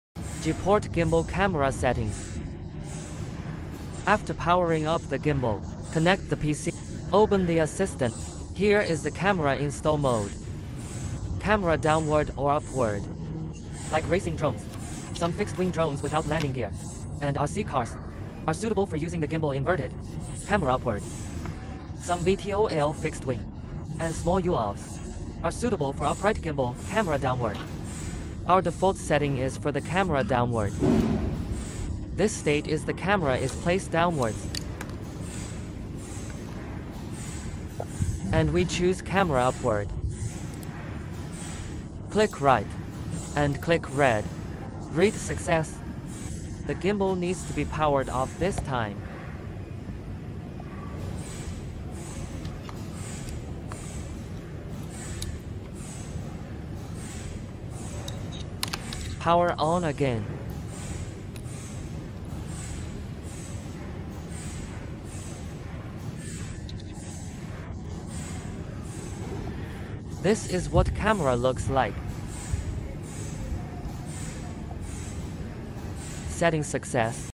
G-Port Gimbal Camera Settings#gimbal
This video will talk about the camera downward and upward modes of the G-port gimbal. What modes are recommended for different drones and model cars
If you do not have G-Port Assistant software, you can contact customer service to geti it.

If you are using Windows but cannot install or use the Assistant software, you can check whether your computer is missing a driver.